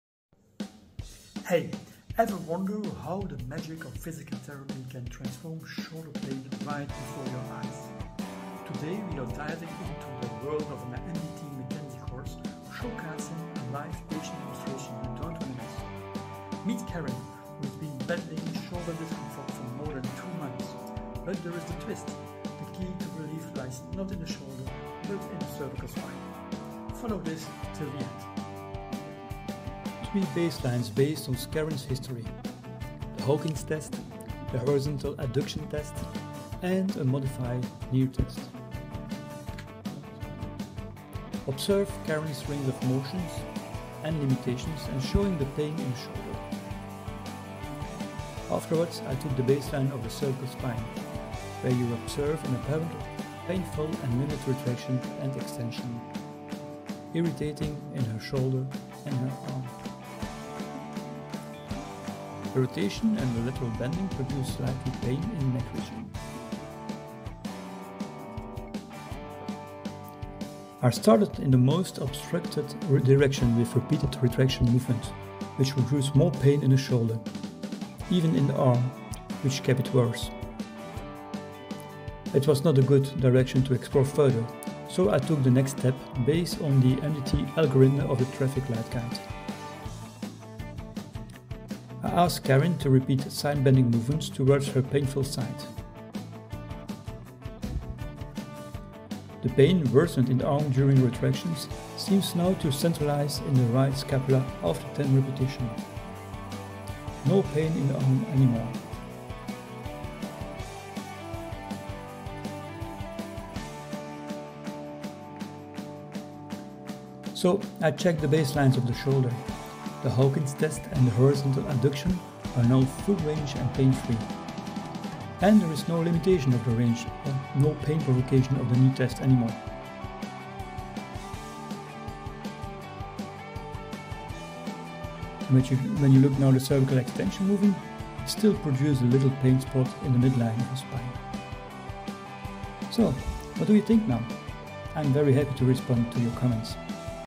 🌟 Transforming Shoulder Pain with MDT McKenzie: A Live Patient Demonstration 🌟
Hey everyone!

Have you ever marveled at how physical therapy unveils its magic, transforming pain into relief right before your eyes? Today, we're excited to share something special from the world of the MDT McKenzie course—a captivating live patient demonstration that you simply can't afford to miss!

👩‍🔬 Meet Karen, who has been navigating the challenges of shoulder discomfort for over two months. The intriguing twist? The key to her relief traces not to the shoulder itself but to an unexpected hero - the cervical spine!

👀 Watch closely as we guide Karen through meticulously selected movements, tailored to her directional preference. This isn't just about magic; it's grounded in science, and you're about to witness firsthand how these movements initiate visible changes in Karen's shoulder!

🔬 Whether you're a physiotherapy enthusiast, a professional in the medical field, or simply someone who appreciates witnessing extraordinary transformations, this demonstration speaks to you. Let's unravel the mystery of pain relief together, learning how targeted movements can forge tangible results.

The transformation - from before to after - will astonish you!

We delved deep, starting with three baselines based on Karen's history: the Hawkins test, the Horizontal Adduction test, and a modified Neer Test. Observing Karen's range of motion limitations was just the beginning.

We navigated through cervical spine baselines as we progressed, revealing significant insights into Karen's condition and the path to relief. With every movement, we saw changes - some directions led to more pain, while others, notably side-bending movements towards the painful side, began to centralize the discomfort, leading to remarkable improvements.

🔍 By the end of our session, tests that initially indicated pain and restriction showed no limitations or pain, highlighting the power of targeted physical therapy techniques.

Are you curious about the science behind the transformation or have thoughts to share? For more enlightening details, dive deeper into our journey and join the conversation by heading to our Blog, McKenzie Belgium.

We're eager to hear your thoughts and answer any questions you might have!